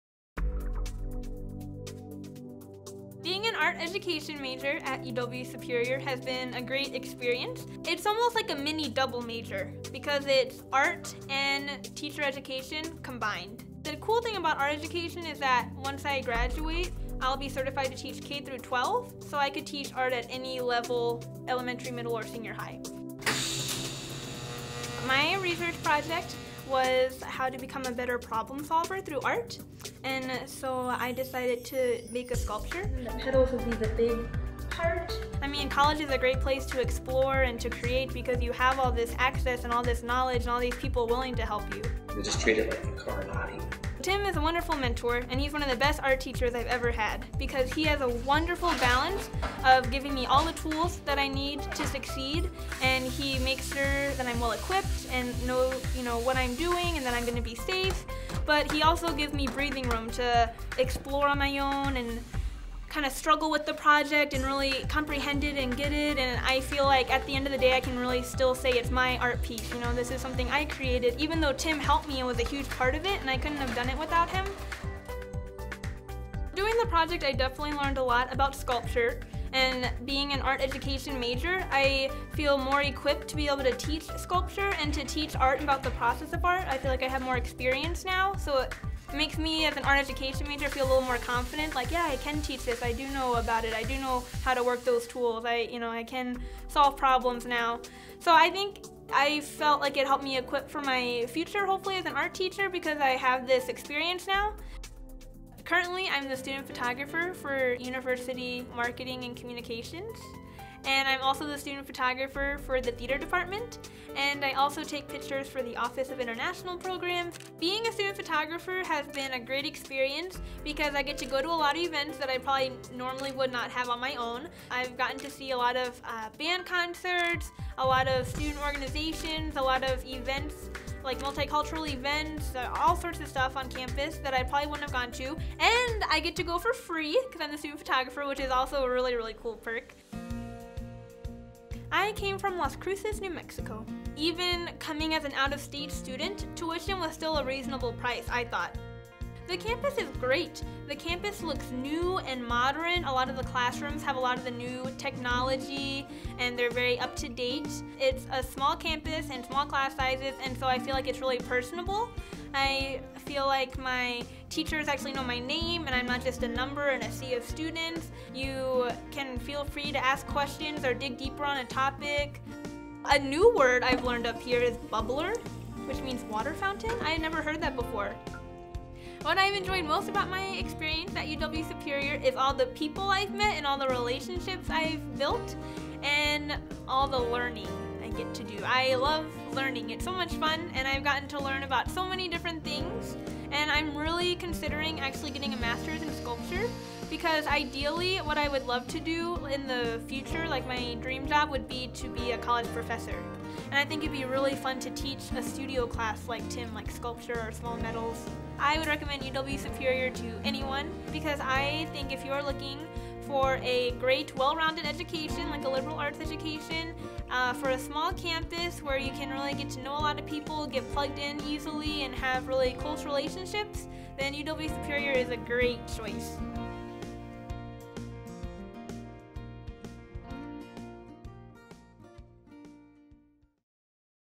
Mariel Santos | Student Profile
Meet Art Education major Mariel Santos.